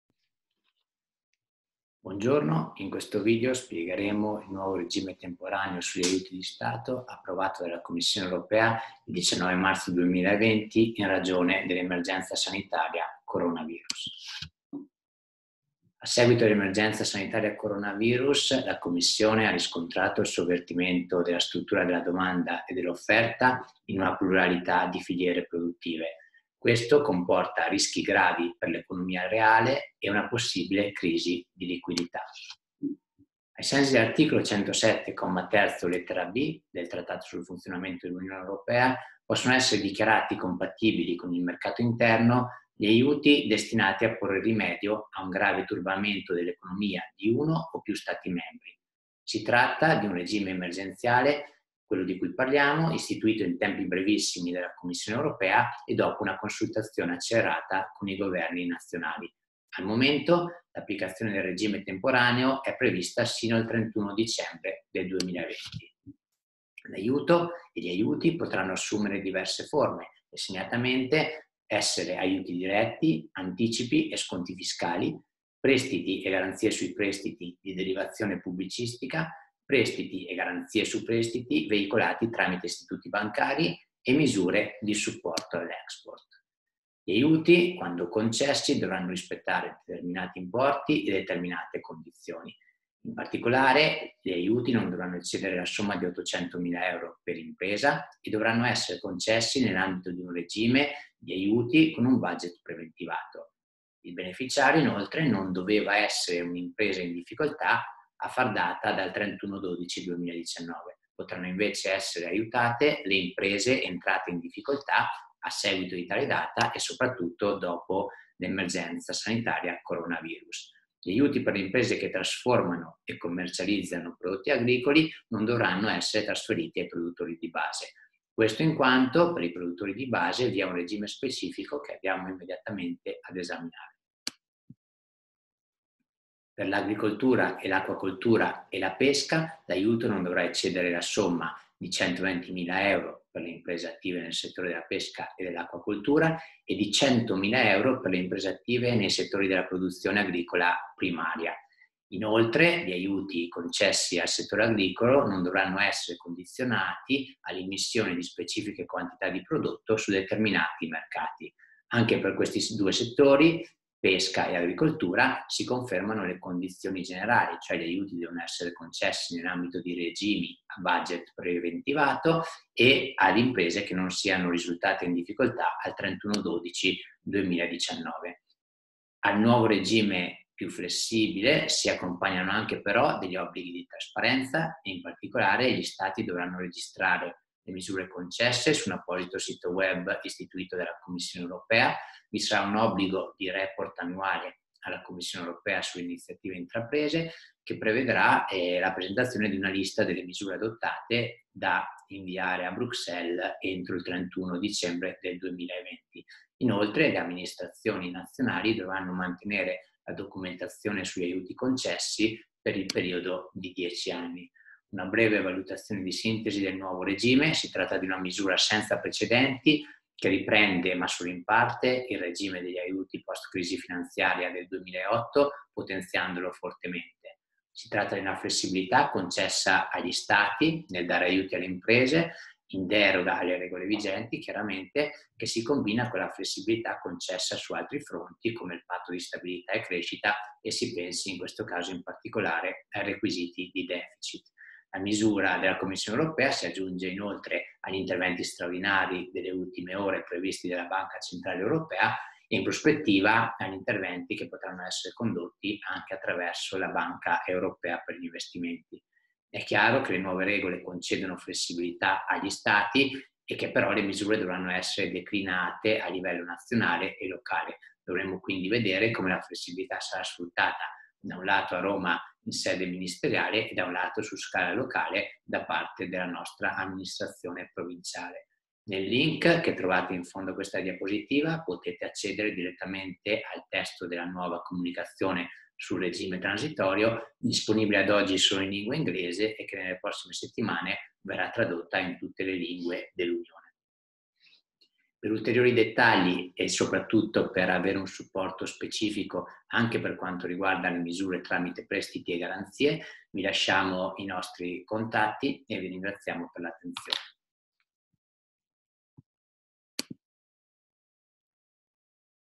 Covid-19, la Commissione europea concede flessibilità sugli aiuti di Stato
Emergenza virus Covid-19, la Commissione europea concede flessibilità sugli aiuti di Stato
Le conseguenze economiche dell’emergenza Corona virus costringono la Commissione Europea a modificare temporaneamente la normativa che regola la materia degli aiuti di Stato. Via libera al sostegno pubblico all’economia.
Alessandro Ceschi (federcoop): “è una misura senza precedenti, che riprende e potenzia il regime introdotto con la crisi finanziaria iniziata nel 2008. La flessibilità concessa agli Stati può combinarsi con la flessibilità anche su altri fronti, come il Patto di stabilità e crescita”.
L’ufficio di Bruxelles della Federazione disponibile per approfondimenti